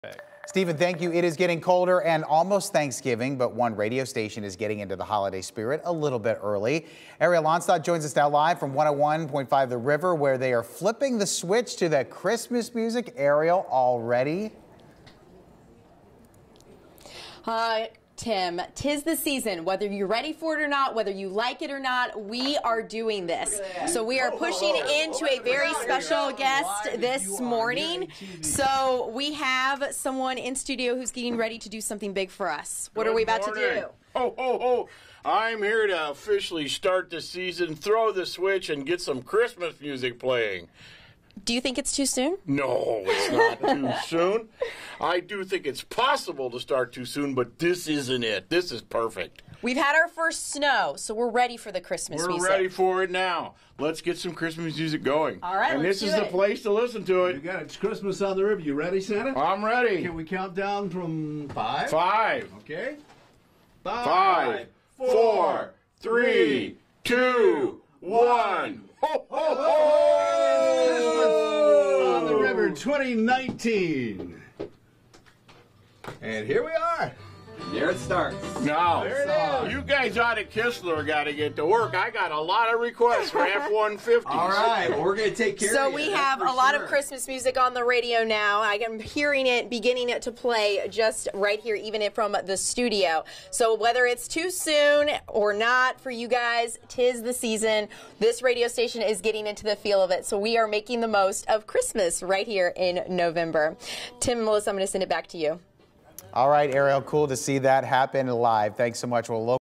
The switch is flipped: The River now playing all Christmas music, all the time | Go 419
Santa says it's not too soon for Christmas music on The River 101.5 in Toledo.